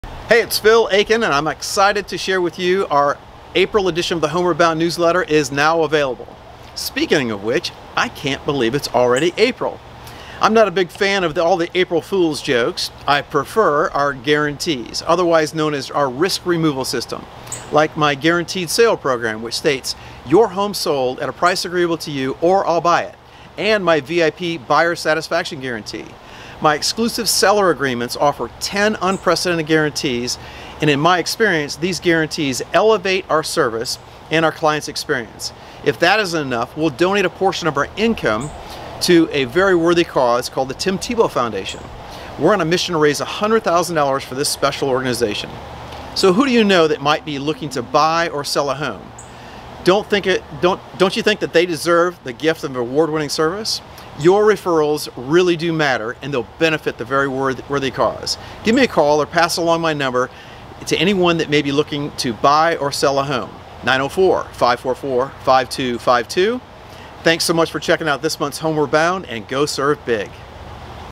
Give Me ONE Minute and We’ll Save a Kids Life Together…
Hello, it's Phil Aitken from Your Home Sold Guaranteed Realty - The Phil Aitken Home Team.
I just put the finishing touches on this month’s Newsletter - REAL ESTATE NEWS FROM ME, PHIL AITKEN.

In this month’s issue:
*How I was April Fooled.
*One minute is about all it takes to help save a kid’s life.